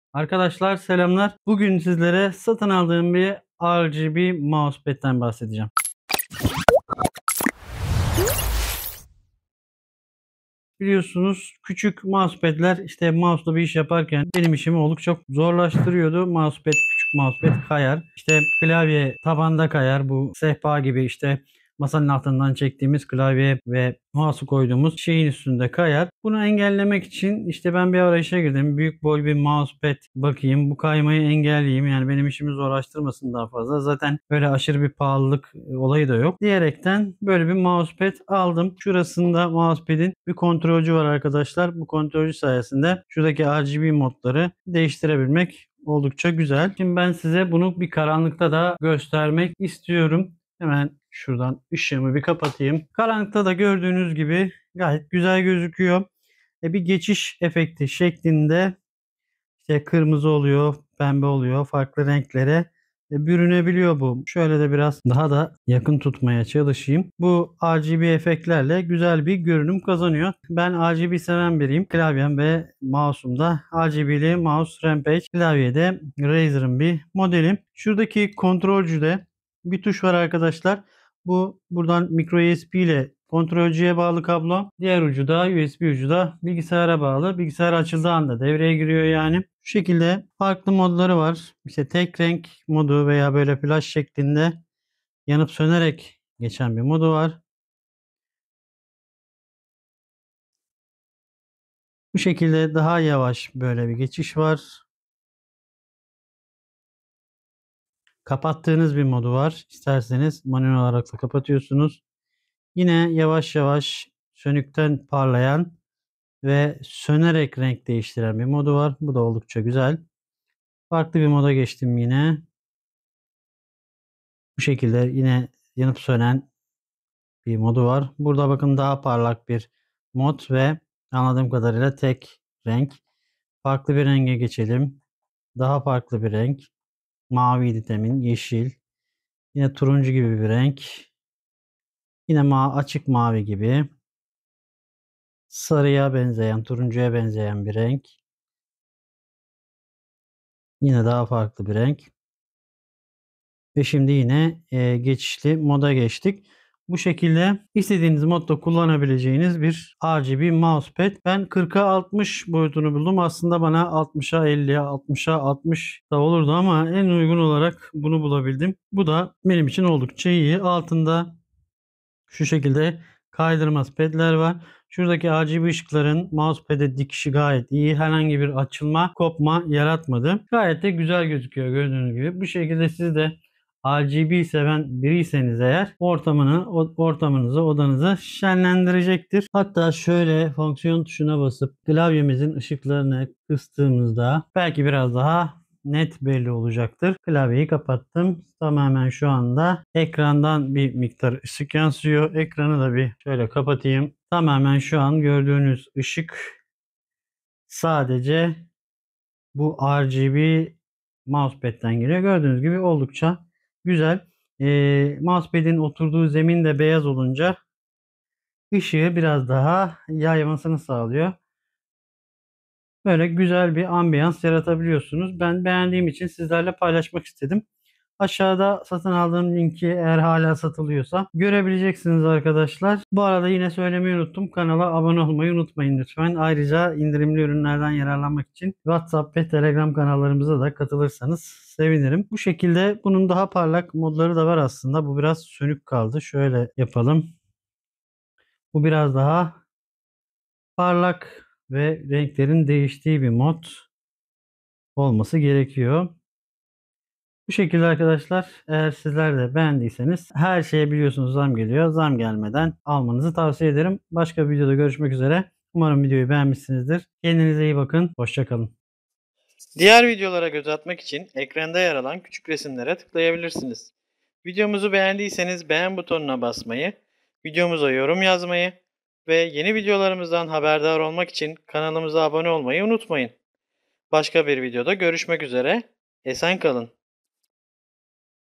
IŞIKLI MIŞIKLI RGB MOUSE PAD İNCELEMESİ
Ürün Linki:
Bu videoda ışıklı RGB mouse pad inceliyoruz.

Youtube Kanal Düzenleme, İçerik Üretimi  ve İnternetten Para Kazanma Konulu

Kişisel Bulut Depolama - Nas Cihazı - Otomatik Dosya Yedekleme - PC